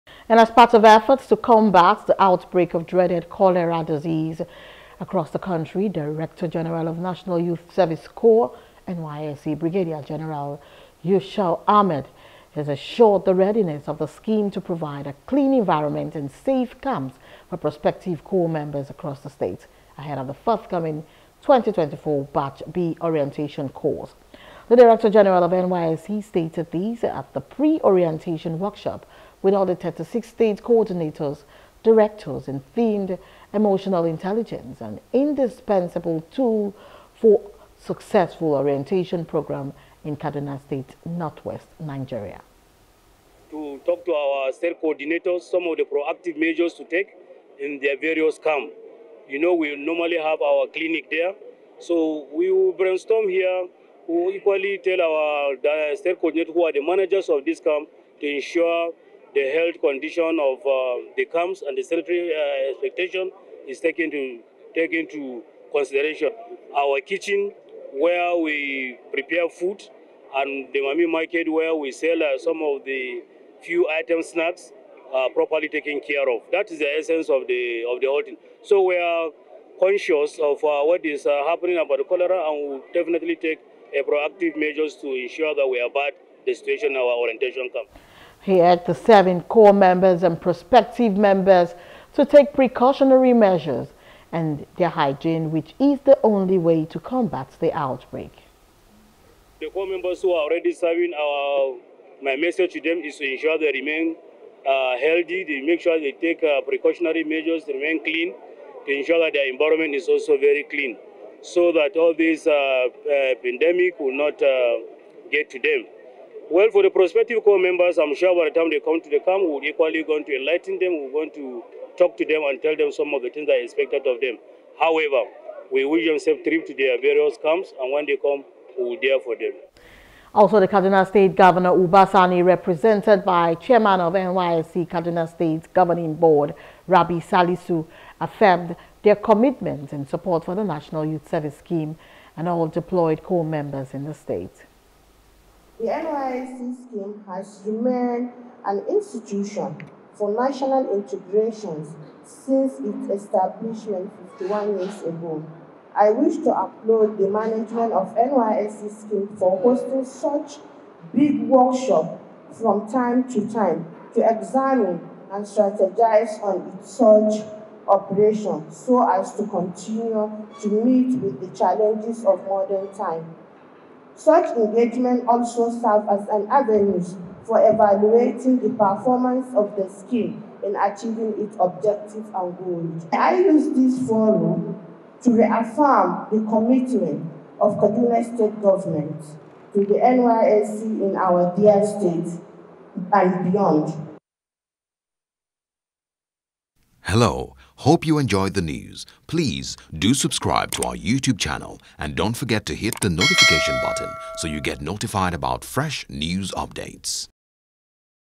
N Y S C D.G. Assures Clean Environment, Safe Orientation Camps For Corp Members
As part of efforts to combat the outbreak of dreaded cholera disease across the country, Director General of National Youth Service Corp,(NYSC) Brigadier General Yusha'u Ahmed, has assured the readiness of the scheme to provide a clean environment, and safe camps for prospective corps members across the states, ahead of the forthcoming 2024 batch B orientation course.